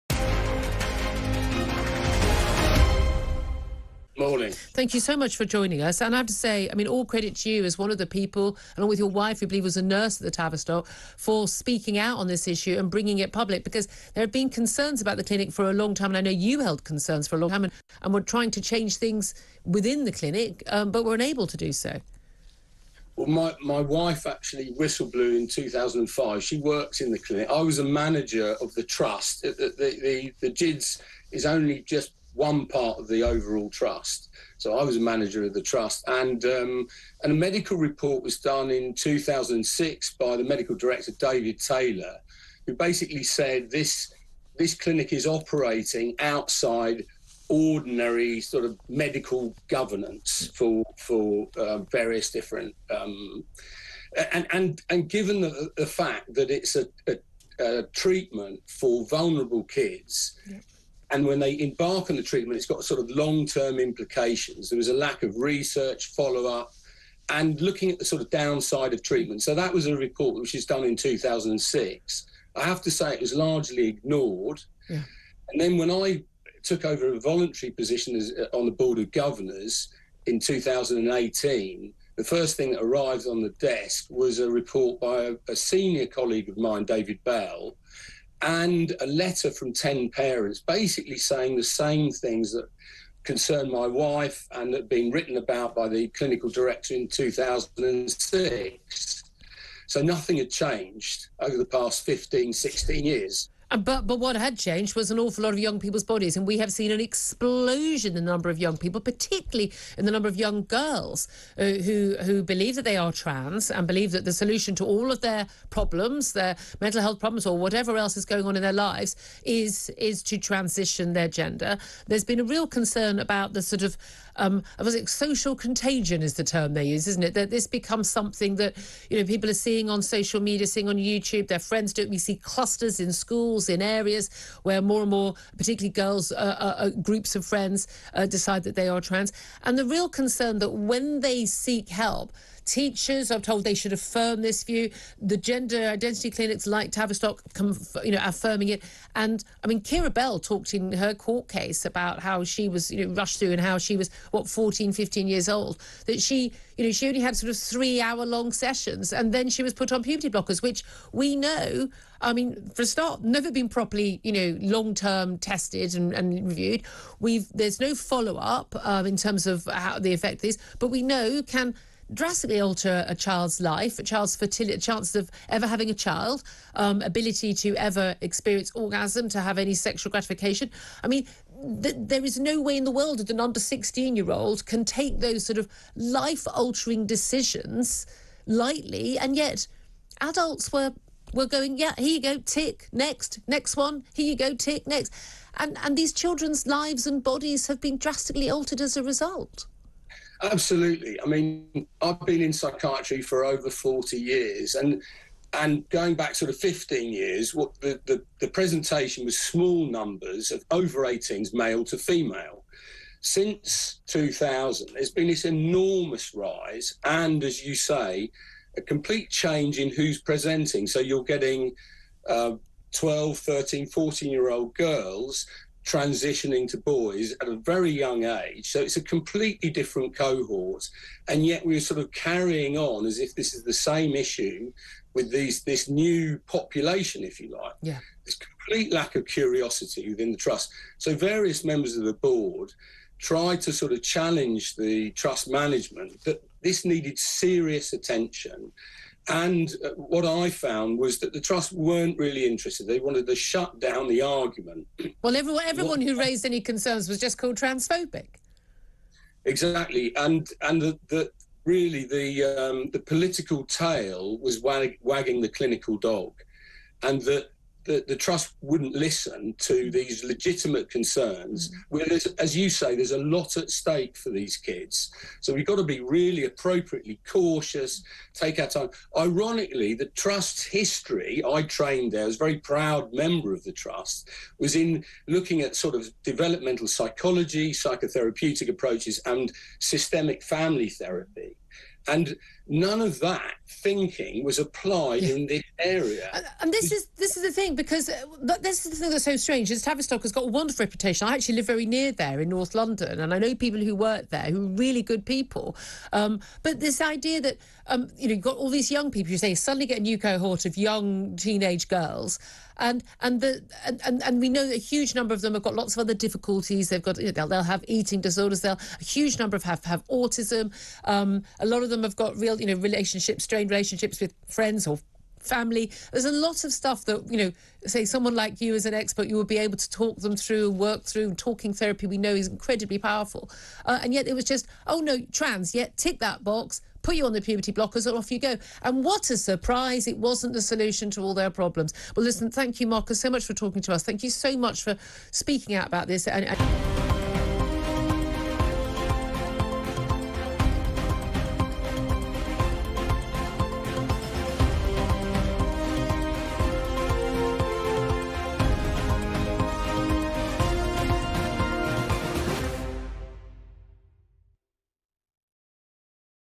Ex clinical lead at Tavistock transgender clinic slams trust
Marcus Evans, ex clinical lead at the Tavistock transgender Clinic which is now closing, criticises the trust.

It comes as the NHS is closing its gender identity clinic for children and young people and replacing it with a regional network after it was told that only having one provider “is not a safe or viable long-term option”.

The contract for the Gender Identity Development Service (GIDS) at the Tavistock & Portman NHS Foundation Trust will be brought to a close, the NHS said.

It said it intends to build a “more resilient service” by expanding provision into a regional network, and is taking “immediate steps” to establish two services led by specialist children’s hospitals in London and the North West.

The NHS aims for these to be fully up and running by Spring 2023.

It follows recommendations from Dr Hilary Cass, who is leading an independent review into Gender Identity Services for children and young people.